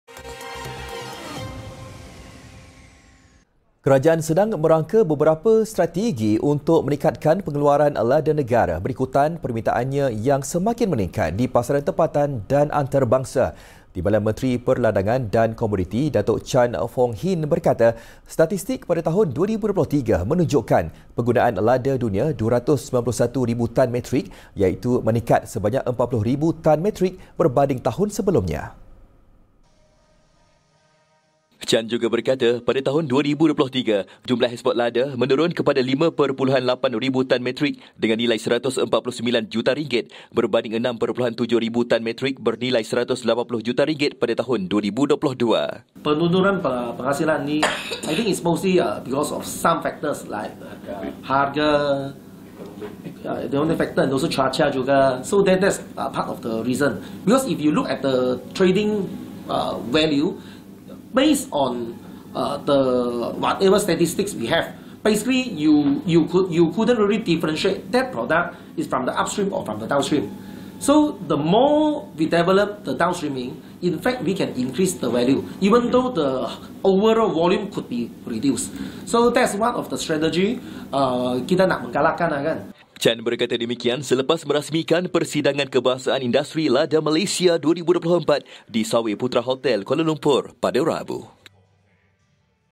16 MEI 2024 | Trend Permintaan Lada Dunia Dijangka Terus Meningkat
Kerajaan sedang merangka beberapa strategi untuk meningkatkan pengeluaran lada negara berikutan permintaannya yang semakin meningkat di pasaran tempatan dan antarabangsa.

Sumber: BERNAMA